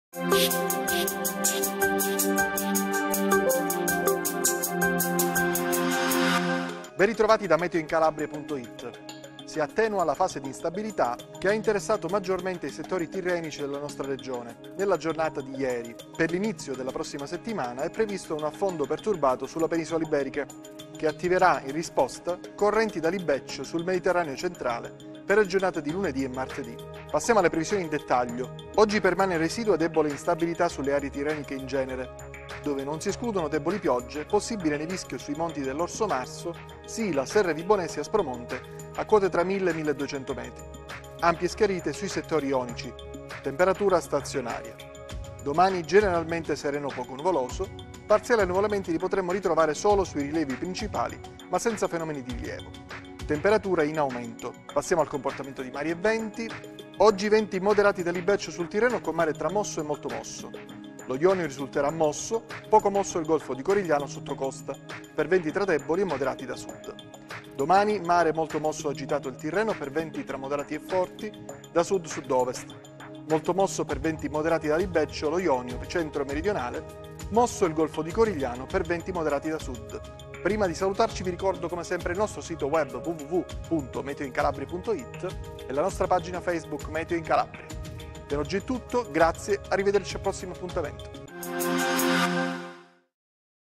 METEO DEL 10 12 17
Previsioni del tempo sulla Calabria per DOMENICA 10 e LUNEDI' 11 DICEMBRE 2017 a cura di
In studio Alfredo De Rose